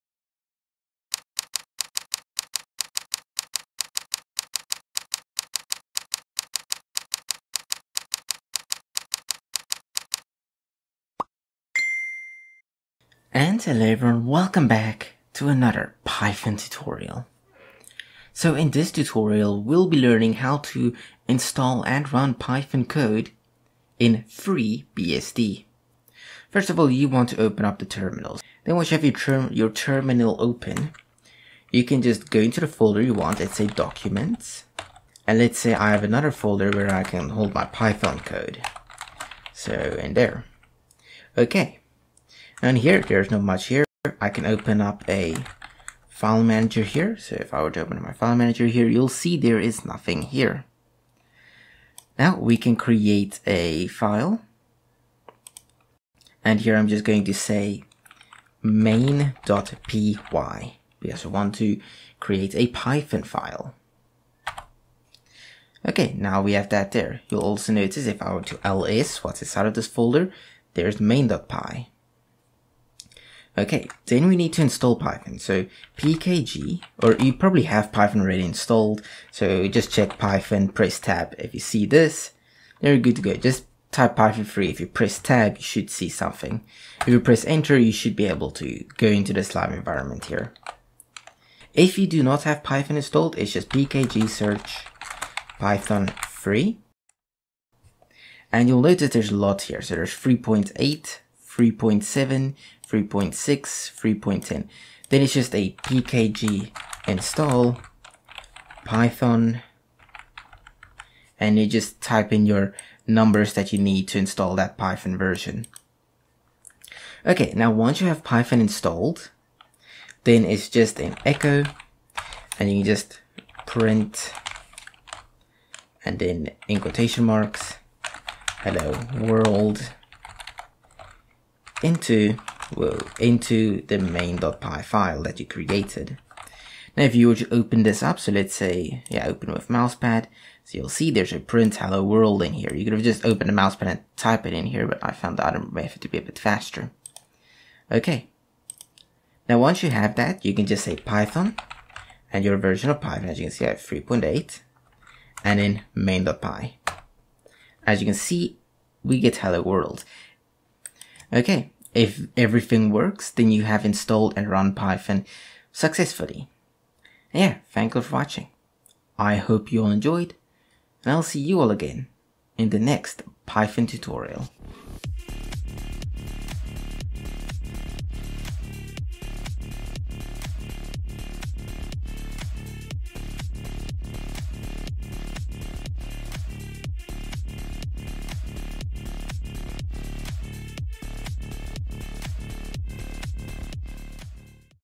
Install and Run Python on FreeBSD - Python Tutorial (Part 4)
Learn how to install Python on your BSD machine!